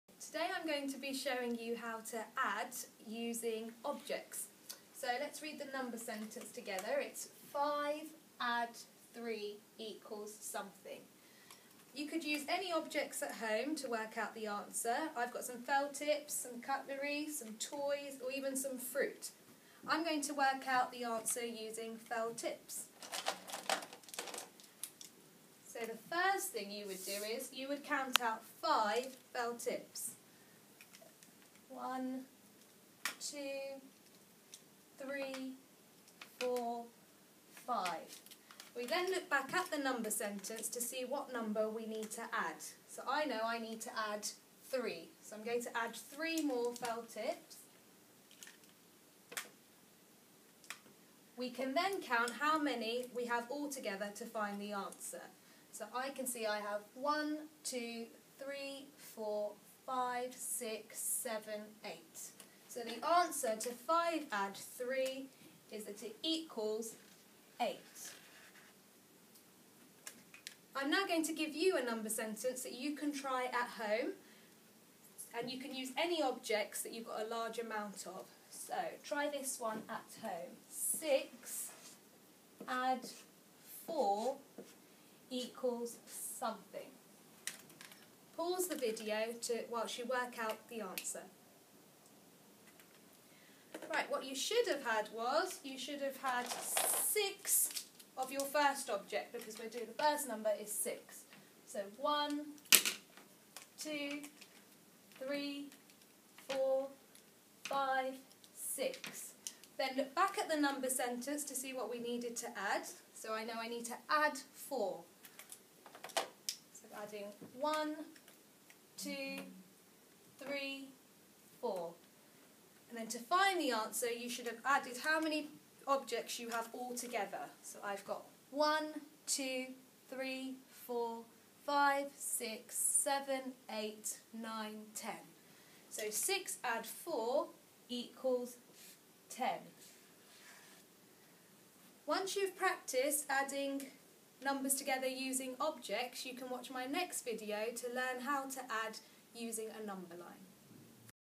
Addition using objects
Watch this video to learn how to add two numbers together using objects.